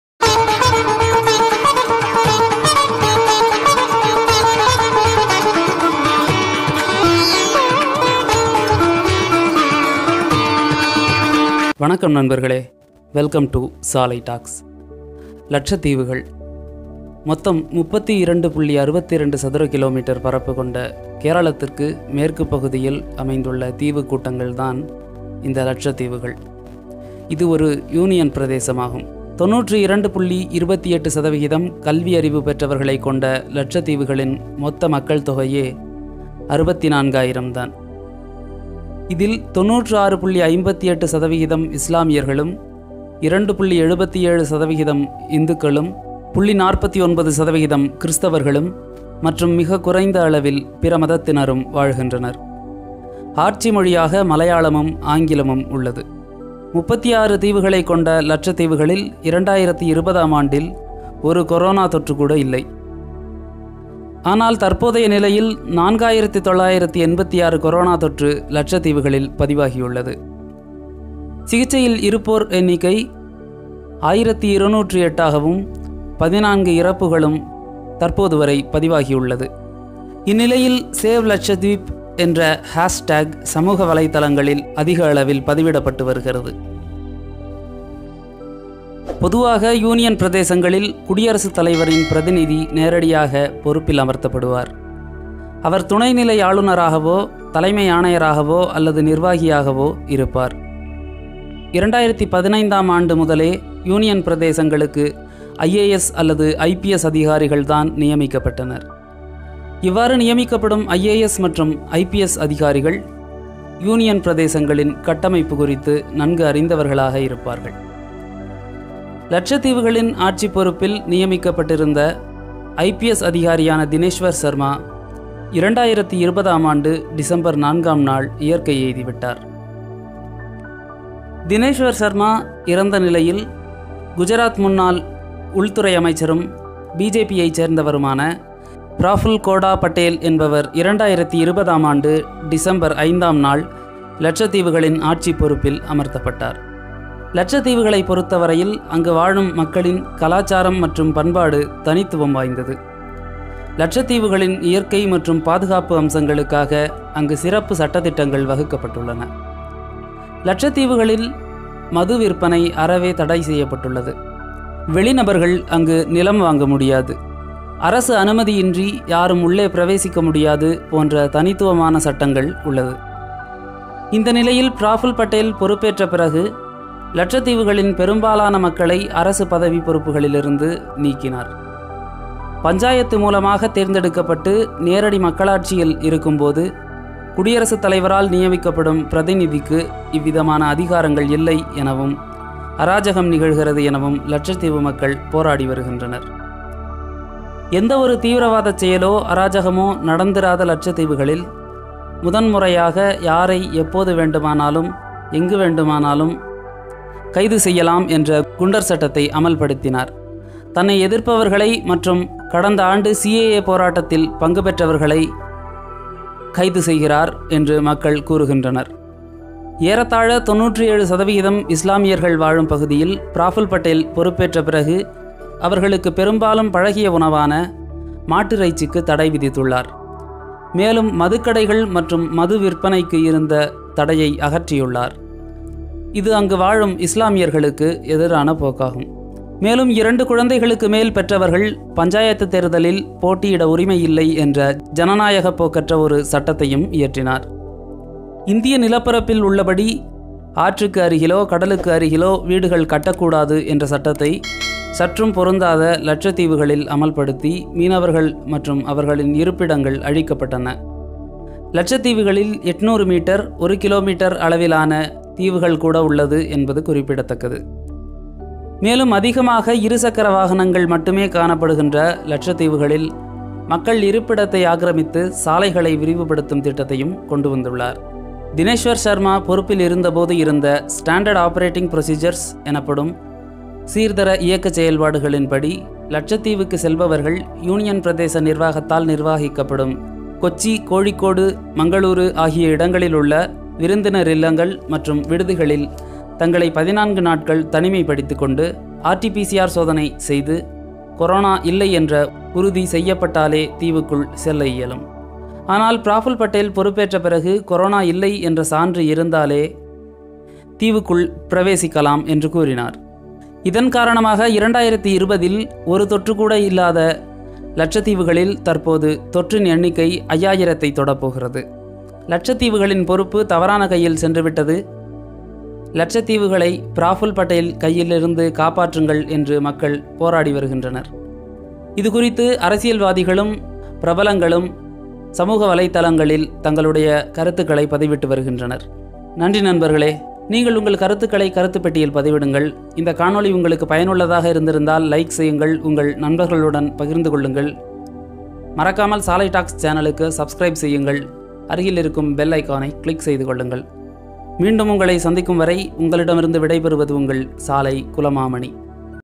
Save Lakshadweep - Lakshadweep problem explained in Tamil | Salai Talks
Save Lakshadweep - Lakshadweep problem explained in Tamil
In this video i have explained about the problems with the Lakshadweep administration officer and about Lakshadweep hashtag

Please comment your valuable words in comment box.